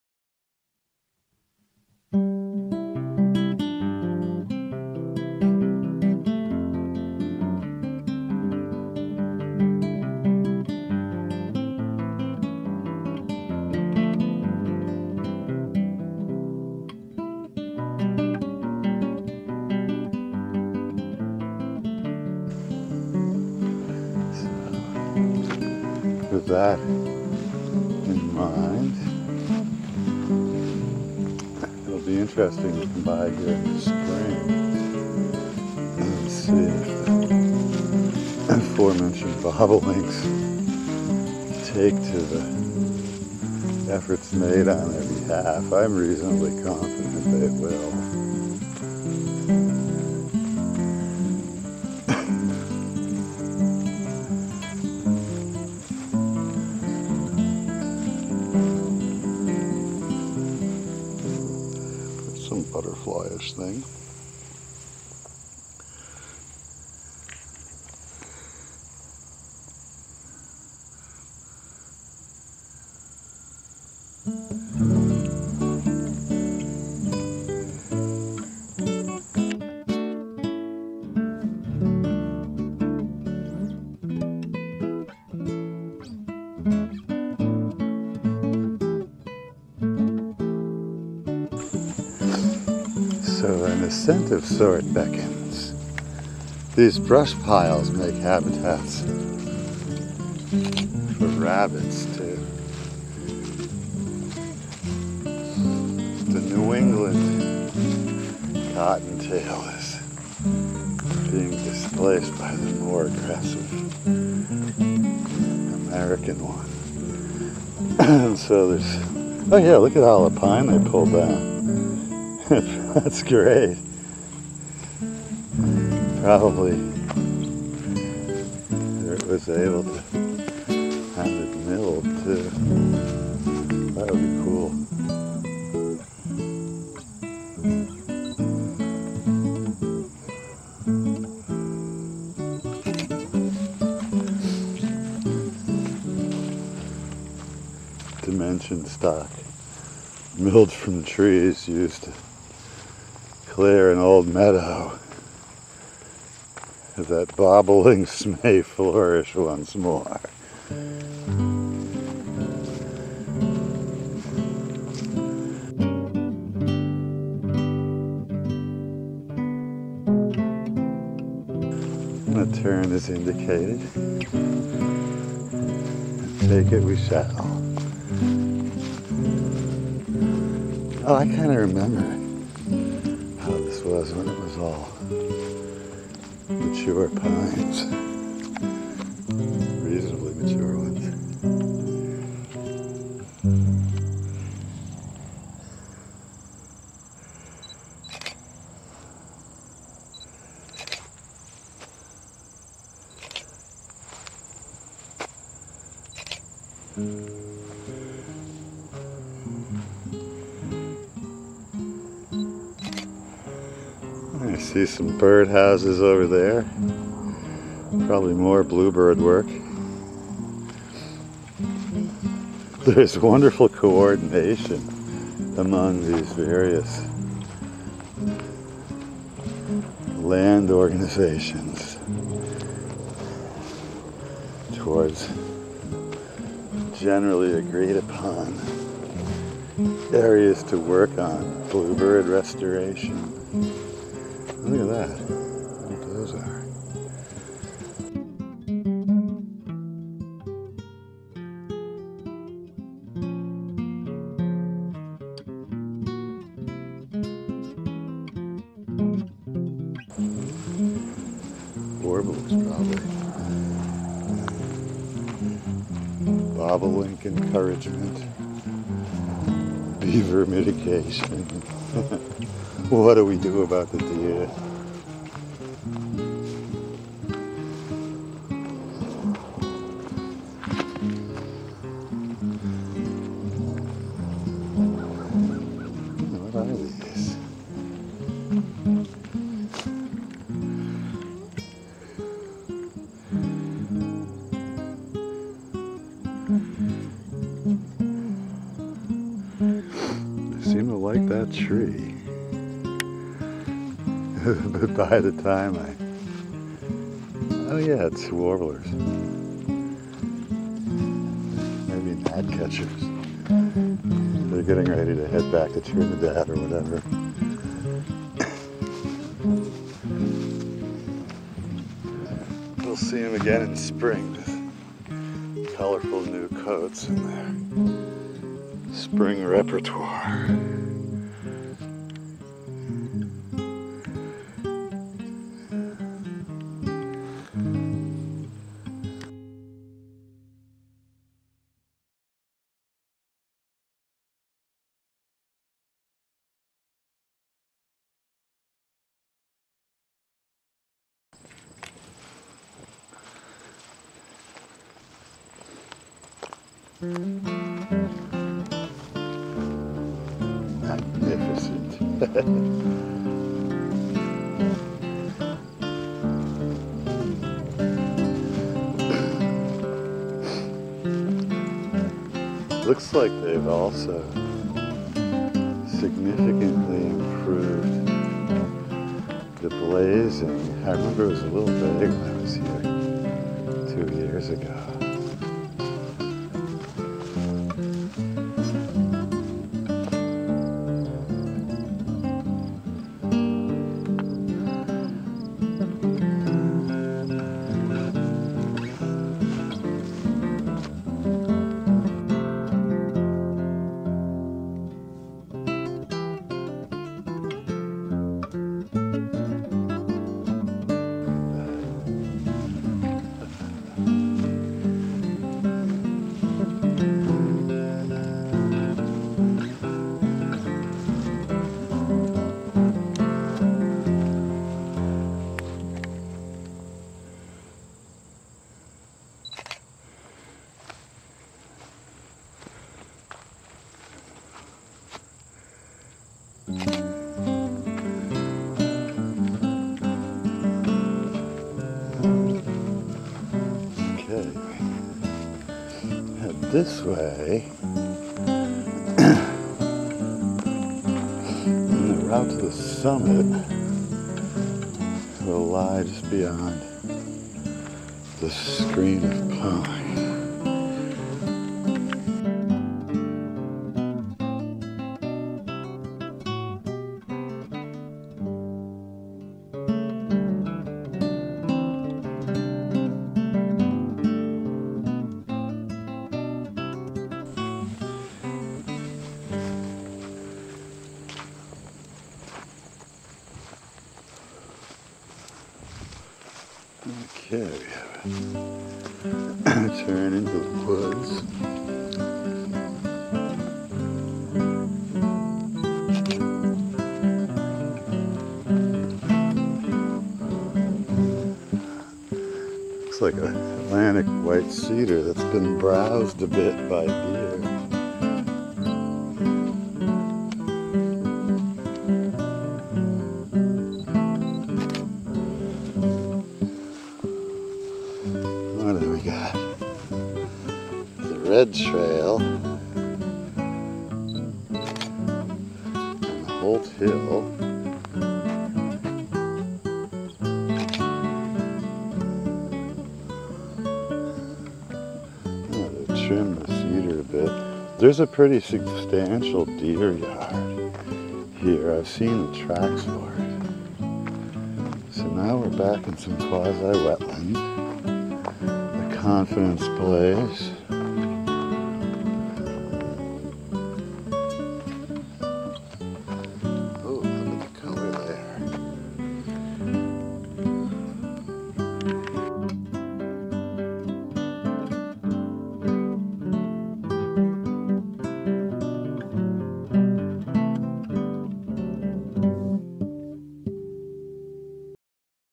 Bay Circuit Trail : Andover MA Ward Reservation Part 2.
This segment covers the bold new habitat restoration area where a meadow is being returned to the mix.